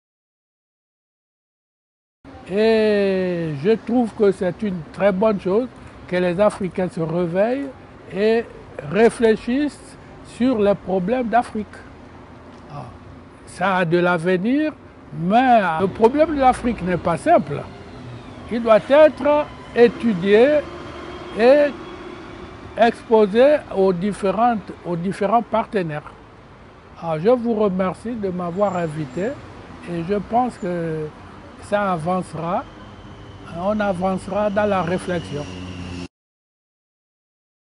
C'est une bonne chose que les africains se réveillent (Dr Biffot)
Rencontre Estivale de la Fédéralitude
Témoignages de quelques participants